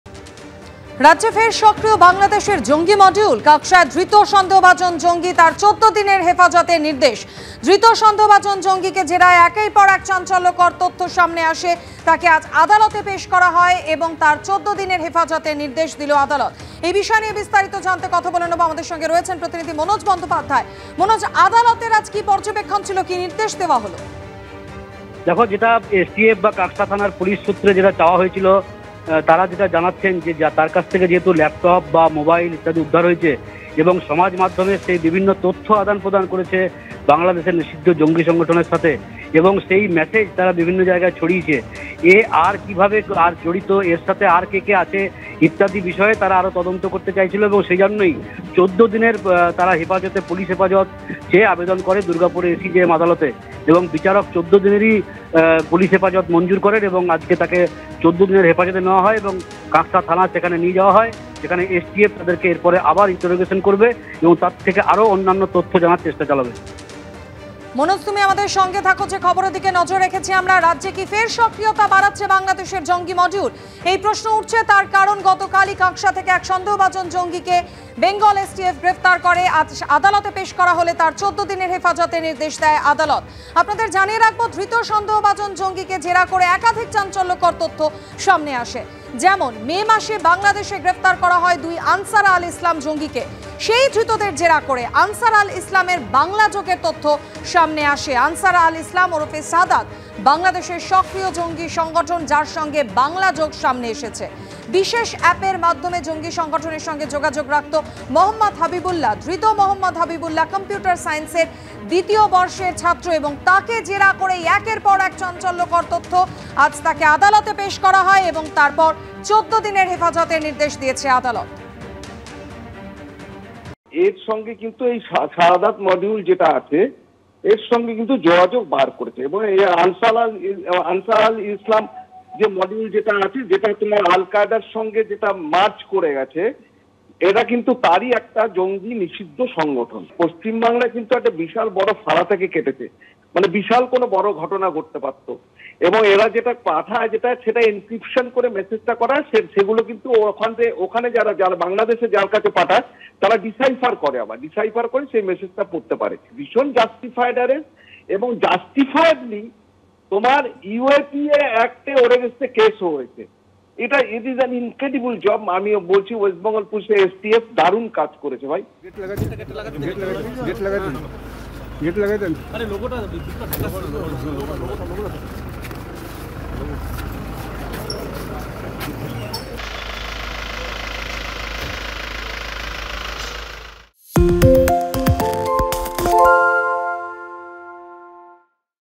Kaksha Terrorist Arrest: রাজ্যে সক্রিয় বাংলাদেশের জঙ্গি? সন্দেহভাজন জঙ্গিকে জেরায় চাঞ্চল্যকর তথ্য।
ABP Ananda Live: রাজ্যে ফের সক্রিয় বাংলাদেশের জঙ্গি মডিউল? কাঁকসায় ধৃত সন্দেহভাজন জঙ্গিকে জেরায় চাঞ্চল্যকর তথ্য। 'মে মাসে বাংলাদেশে গ্রেফতার ২ আনসার-আল-ইসলাম জঙ্গি'। 'ধৃতদের জেরায় আনসার-আল-ইসলামের বাংলা-যোগের তথ্য'। 'আনসার-আল-ইসলাম ওরফে শাহদাত বাংলাদেশের সক্রিয় জঙ্গি সংগঠন'। শাহদাতের বাংলা মডিউলের মাথা ধৃত মহম্মদ হাবিবুল্লা: এসটিএফ সূত্র। 'বিশেষ অ্যাপের মাধ্যমে জঙ্গি সংগঠনের সঙ্গে যোগাযোগ রাখত হাবিবুল্লা'। ধৃত মহম্মদ হাবিবুল্লা কম্পিউটার সায়েন্সের দ্বিতীয় বর্ষের ছাত্র। পাকিস্তান-সহ বিশ্বের নানা প্রান্তে রয়েছে শাহদাতের মডিউল: এসটিএফ সূত্র। ধৃত মহম্মদ হাবিবুল্লাকে জেরা বেঙ্গল এসটিএফের, ইউএপিএ ধারায় মামলা রুজু। পরীক্ষার আগে পড়ুয়াদের হস্টেলে বসে প্রশ্ন বিলি! উত্তর মুখস্থ করিয়ে পরীক্ষা দিতে যাওয়ার আগে ছাদে সব প্রশ্নপত্র জড়ো করে আগুন। এবিপি আনন্দর অন্তর্তদন্তে চাঞ্চল্যকর তথ্য।

 / abpanandatv